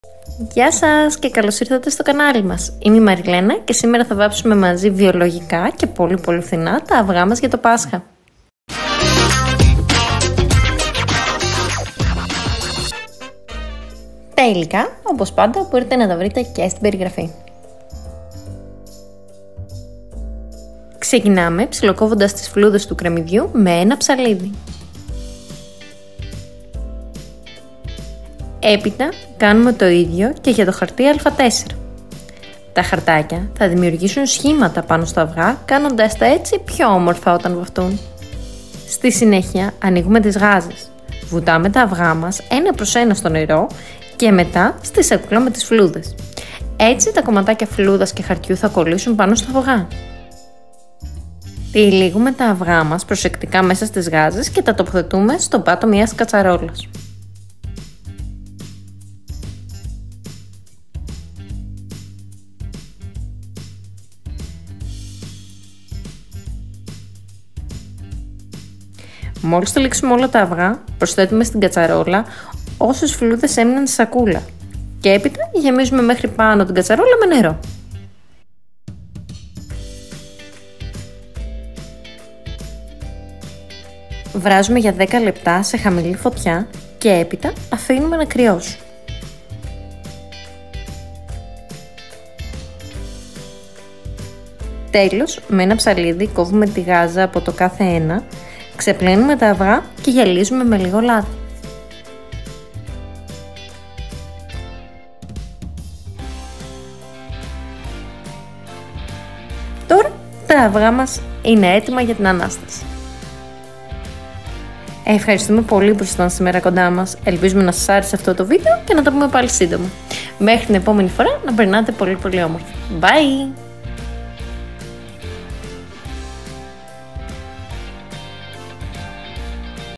ΑΥΓΑ ΒΑΜΜΕΝΑ ΜΕ ΦΛΟΥΔΑ ΚΡΕΜΜΥΔΙΟΥ/ Easter eggs painted with onion peels
Καλησπέρα σας και Χρόνια πολλά! Μ. Πέμπτη σήμερα και βάφουμε αυγά με τον πιο οικολογικό και οικονομικό τρόπο που μπορέσαμε να βρούμε.
Τα υλικά που θα χρειαστούμε είναι :
-Αυγά
-Φλούδες κρεμμυδιού
-Νερό
-Χαρτί Α4
-Γάζες

Ελπίζουμε να σας άρεσε αυτό το βίντεο και πριν φύγετε να μας αφήσετε ένα like ή μια καρδούλα στα σχόλια.
Καλή Ανάσταση!

Good evening and happy Easter! Thursday today and we dye eggs in the most ecological and economic way we could find.
The ingredients we will need are:
-Eggs
-Onion peels
-Water
- A4 paper
- Gauze

We hope you enjoyed this video and before you leave, leave us a like or a heart in the comments.
Happy Easter!

❤️facebook/ Paint Love Create ❤️